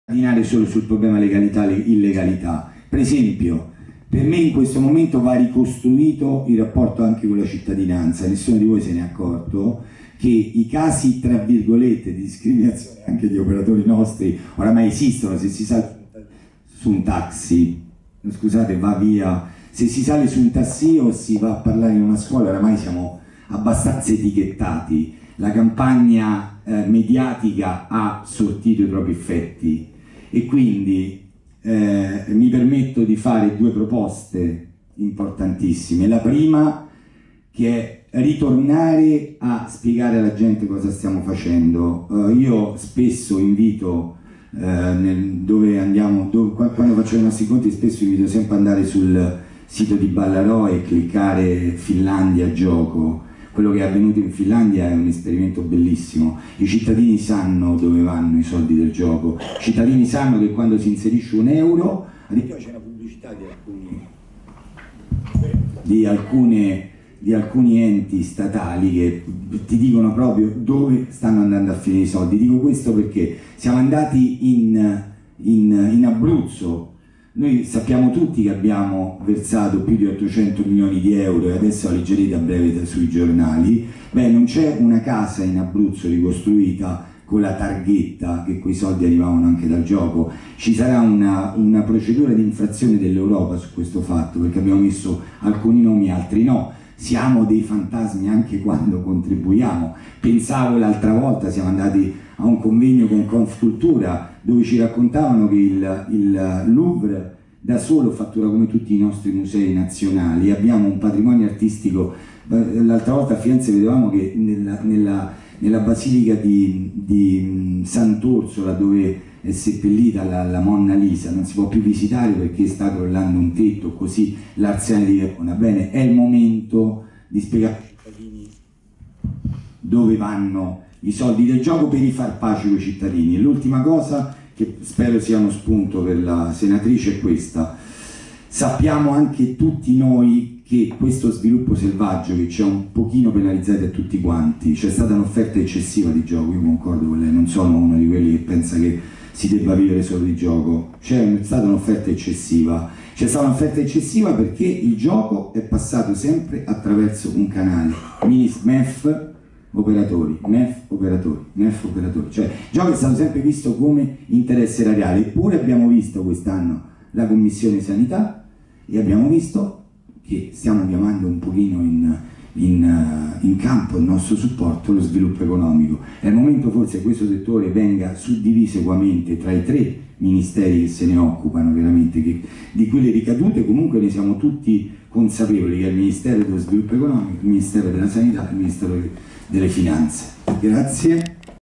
Massimiliano Pucci
Sintesi dell'intervento del presidente ASTRO (SGI) Massimiliano Pucci al convegno “Gioco di contrasto – il Gioco legale è il vero baluardo contro l’offerta illecita di gioco e l’eccesso nel gioco”